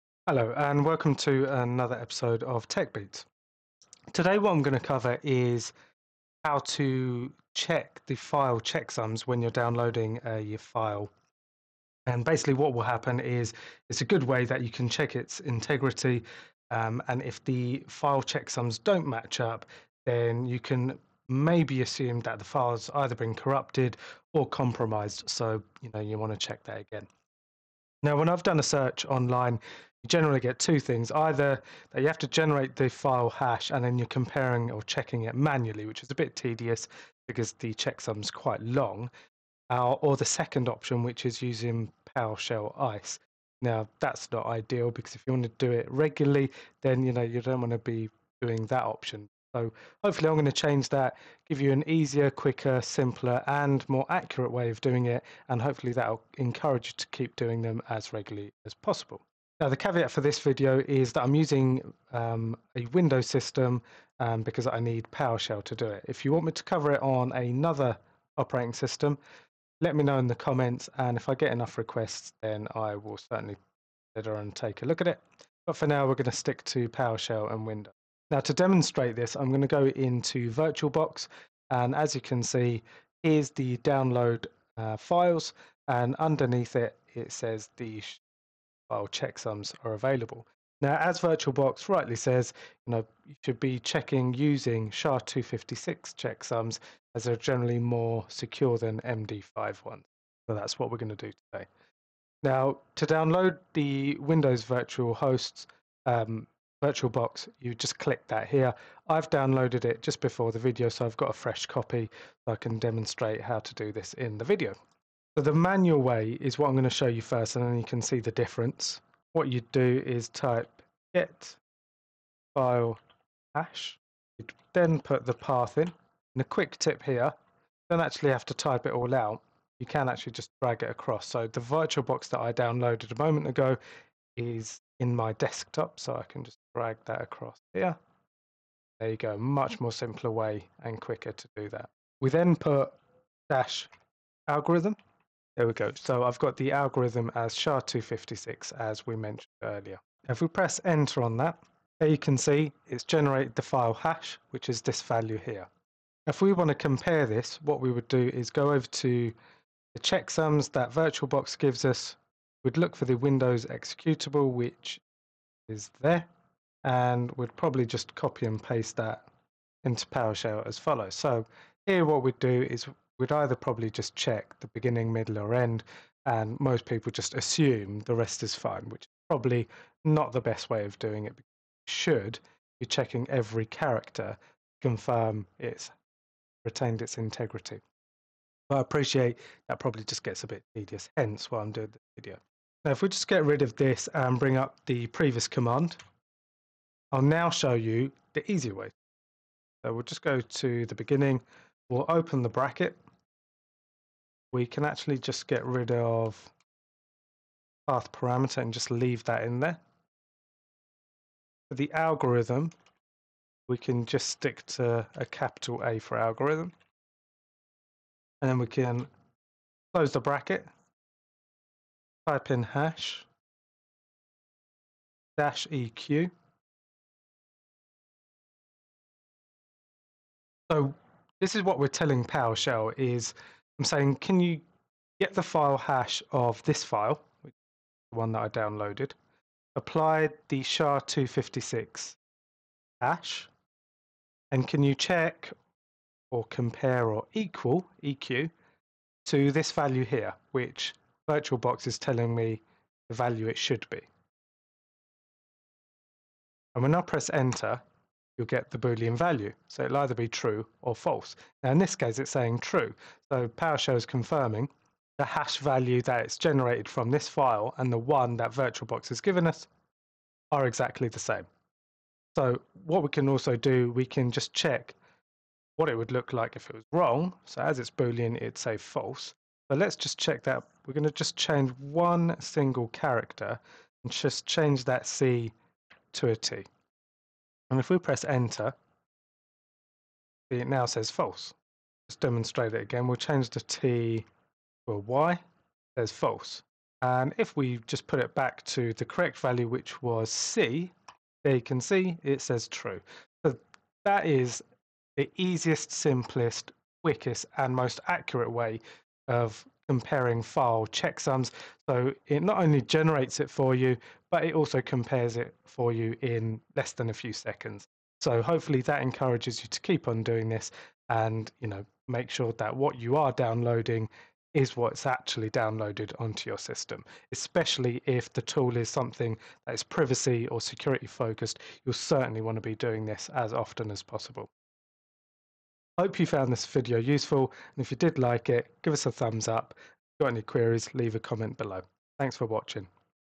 Easiest way to compare file hash and checksums
This video will show you the easiest, quickest and most accurate way of generating and comparing file hashes against checksums using Powershell

No need for Powershell ISE, no need to compare manually and no need to download any software!